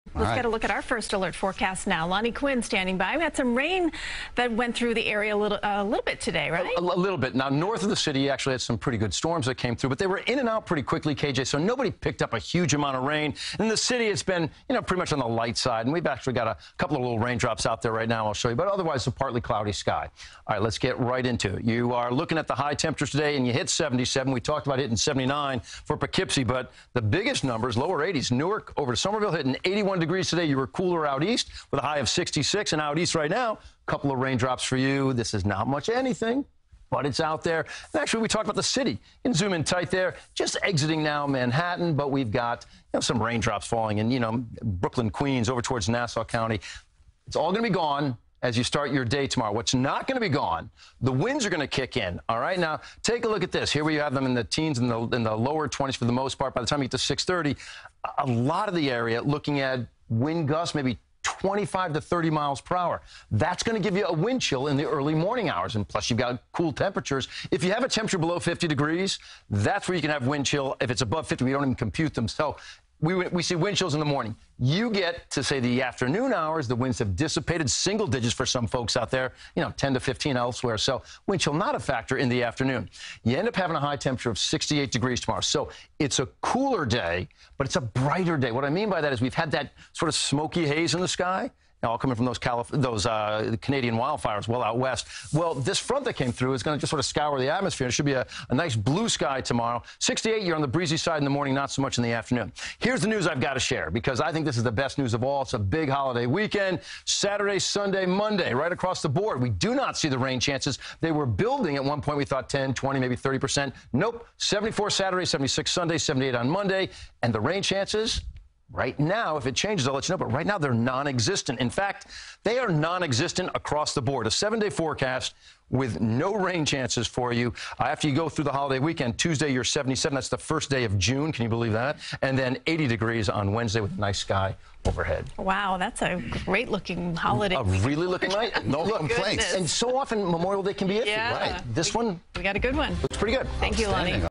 First Alert Weather: Memorial Day Weekend looking better and better
CBS2's Lonnie Quinn says early reports of a possibly wet holiday weekend are being replaced by a lot more sun and temperatures in the upper 70s.